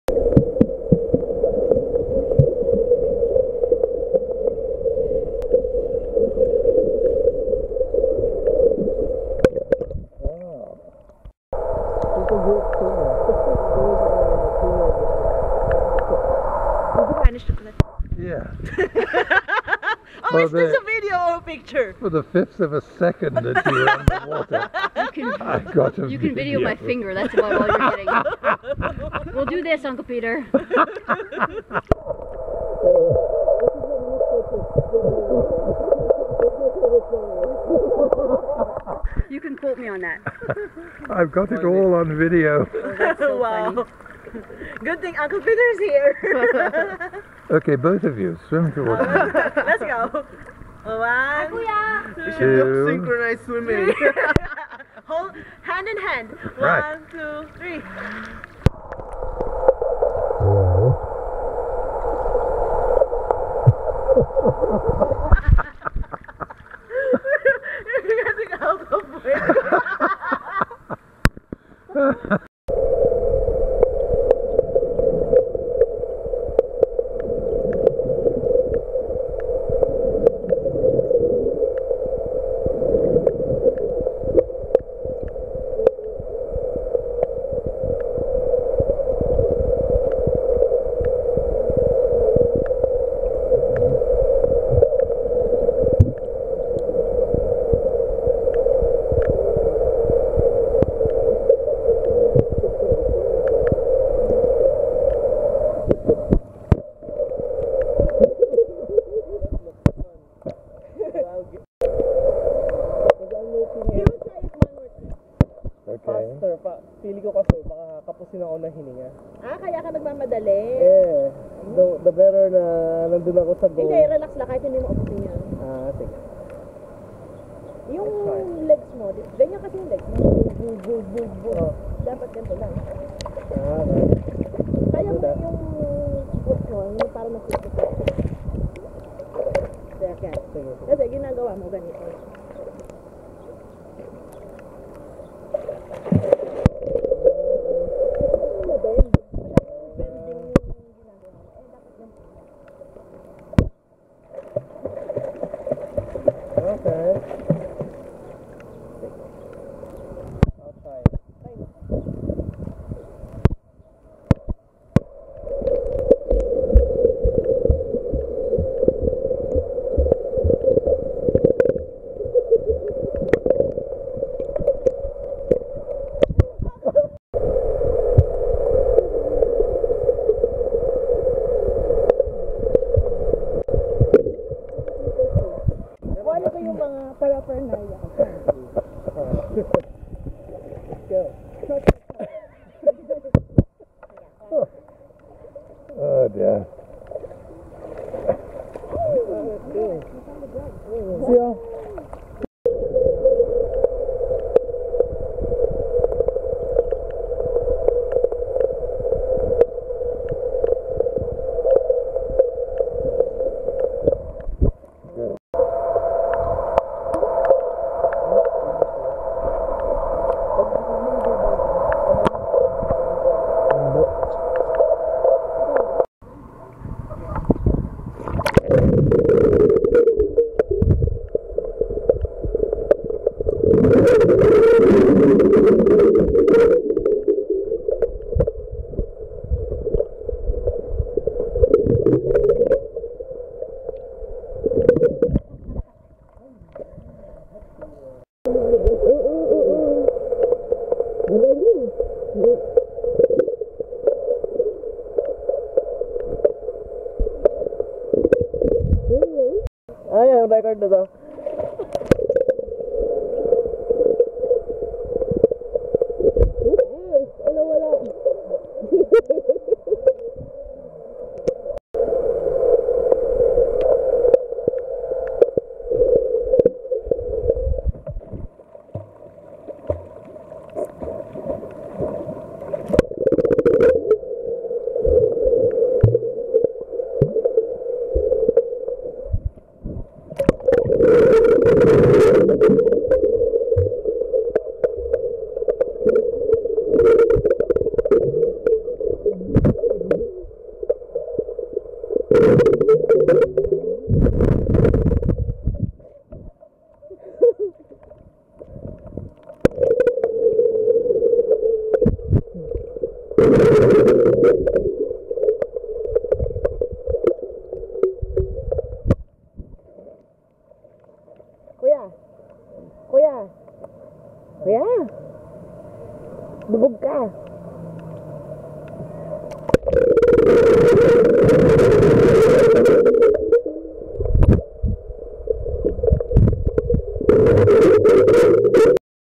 Donnabelle's Zumba Swimming Academy
We were staying at Canyon Cove in Nasugbu, Batangas. Miss Donnabelle Violanda started teaching us to improve our swimming technique, so I suggested she should open an academy with a catchy title.